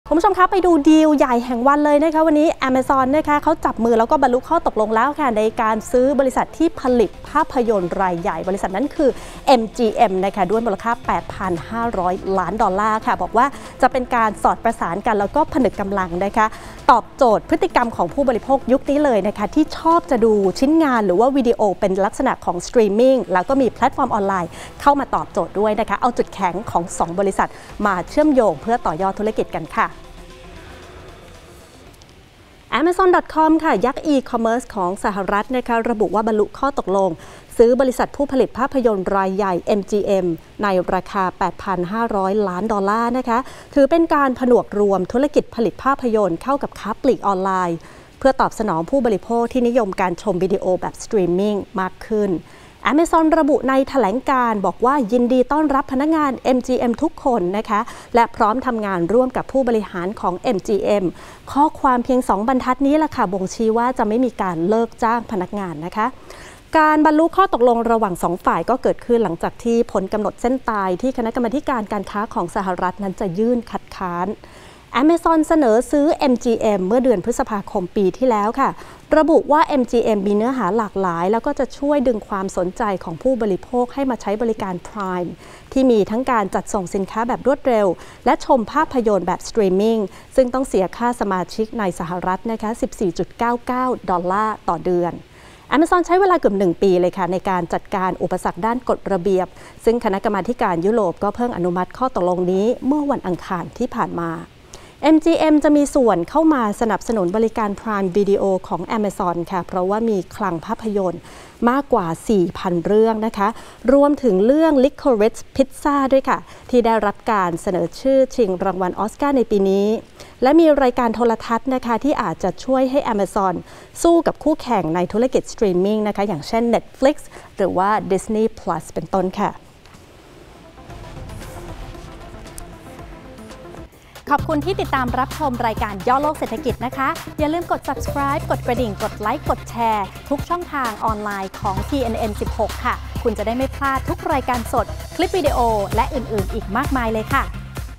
“แอมะซอน” บรรลุข้อตกลงซื้อ “เอ็มจีเอ็ม” 8.5 พันล้านดอลล์ l ย่อโลกเศรษฐกิจ 18 มี.ค.65
“แอมะซอนดอทคอม” บรรลุข้อตกลงซื้อกิจการ “เอ็มจีเอ็ม” ในราคา 8.5 พันล้านดอลลาร์
เสริมแกร่งธุรกิจสตรีมมิ่งช่องทางติดตามสถานีข่าว TNN ช่อง16